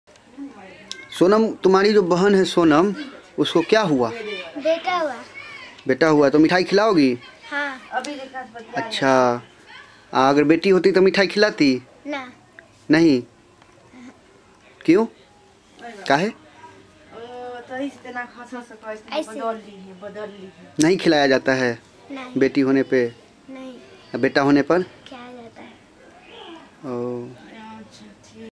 female discrimination
सात साल की बच्ची भी फील करती है स्त्रियों के साथ पक्षपात को